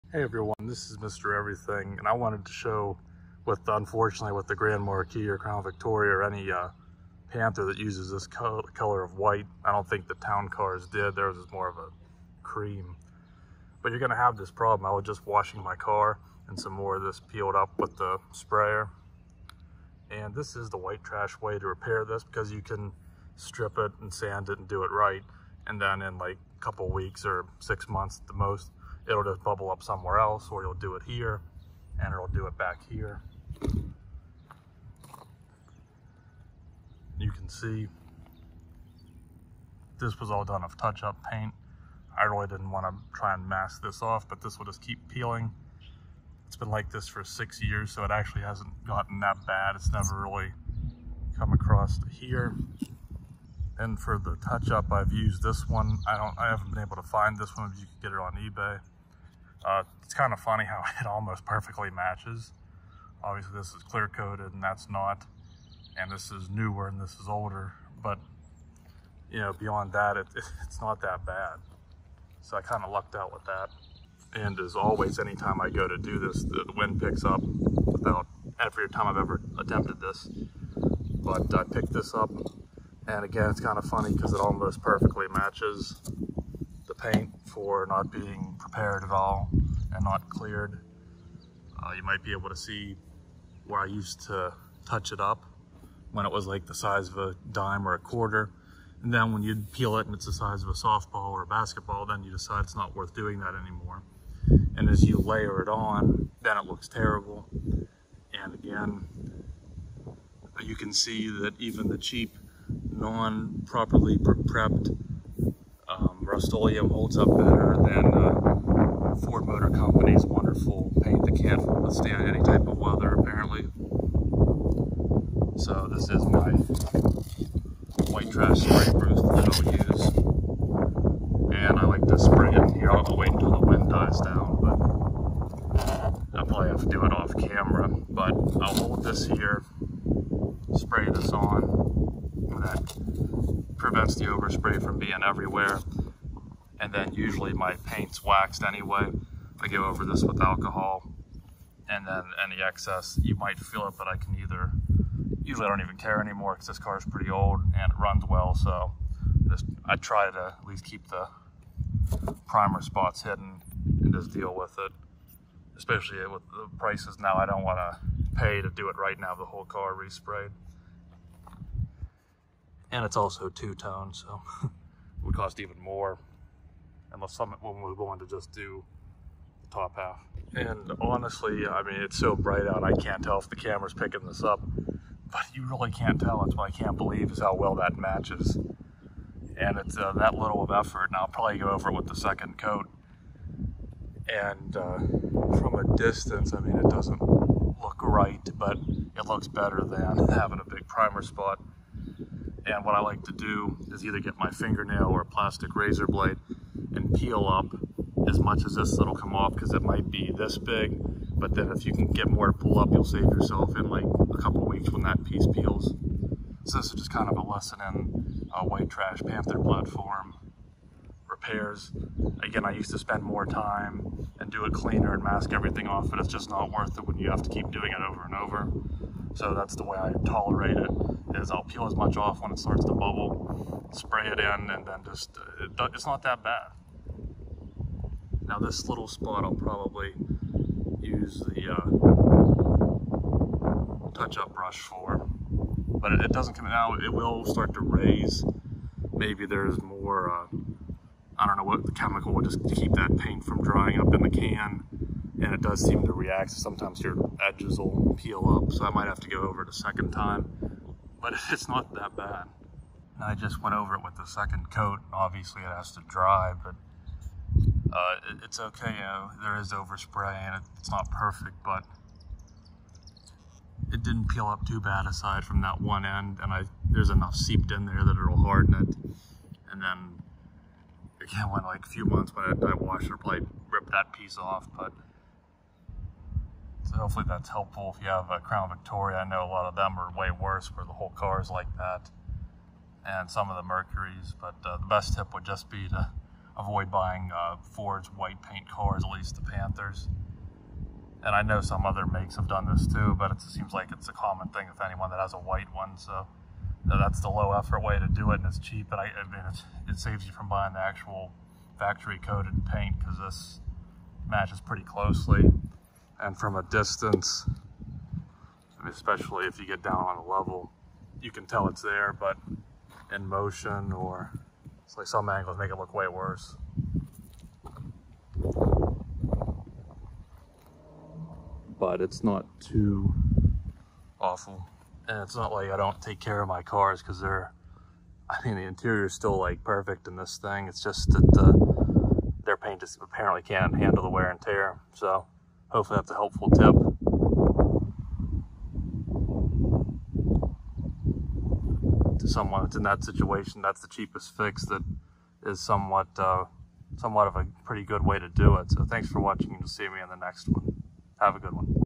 How to Fix Peeling White Paint on a Grand Marquis/Crown Victoria
Anyone who has a factory white paint job on a Mercury Grand Marquis or Ford Crown Victoria (most notably the P71) will notice that overtime the white paint will begin to lose adhesion to the primer and start peeling off. I find it to be a losing battle as I've taken my time to peel back any loose paint, wipe it down with alcohol, and precisely fill it back in with a touch up bottle, only for it to begin peeling somewhere else in a matter of months. This is the current method I use to quickly hide a big primer spot. It doesn't look the best and for a proper fix I would recommend sanding the panel that is peeling down to bare metal, re-primer, paint, and clear if you want to invest the time and money into a better solution.